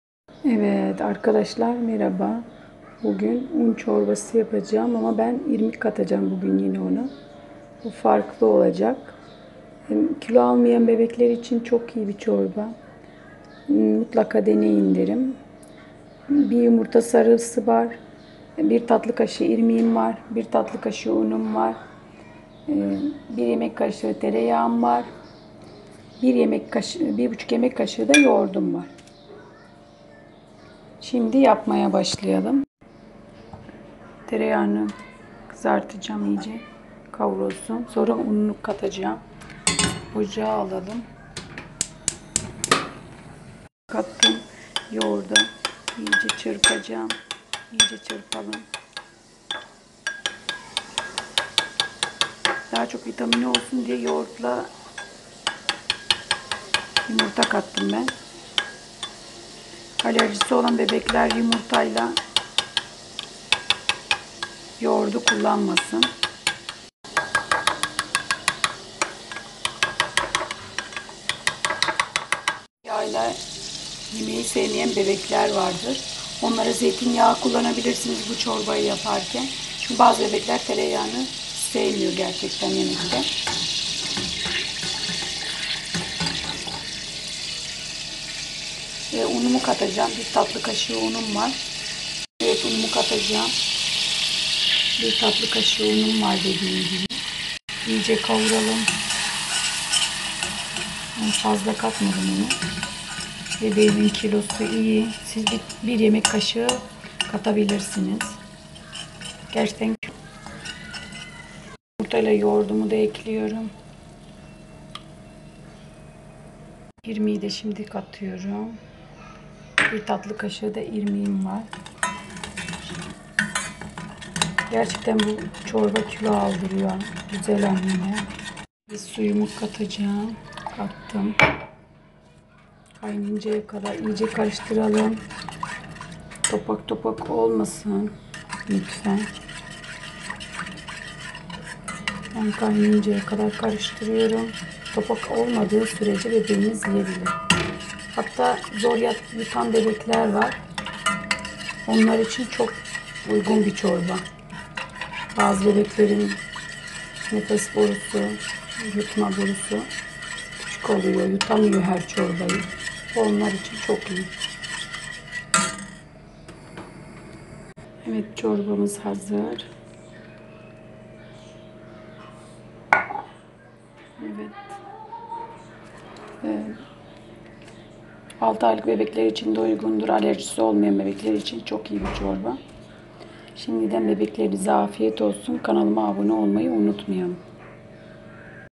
BEBEĞİNİZ KİLO ALMIYORSA MURLAKA BUNU DENEYİN(6-7 AYLIK BEBEKLERE)
1 YUMURTA SARISI
1 YEMEK KAŞIĞI UN
1 YEMEK KAŞIĞI TEREYAĞ
1 TATLI KAŞIĞI IRMİK
1 YEMEK KAŞIĞI YOĞURT
1.5 BARDAK SU
YAPILIŞI:ÖNCE TEREYAĞ ERİSİN 1 YEMEK KAŞIĞI UN KAVURUN 1 YUMURTANIN SARISIYLA 1 YEMEK KAŞIĞI YOĞURTLA BİRLİKTE ÇIRPIN KAVURDUĞUNUZ UN İLE TEREYAĞI EKLEYİN 1 TATLI KAŞIĞI İRMİĞİ EKLEYİN 1.5 BARDAK SU KAYNAYINCAYA KADAR KARIŞTIRIN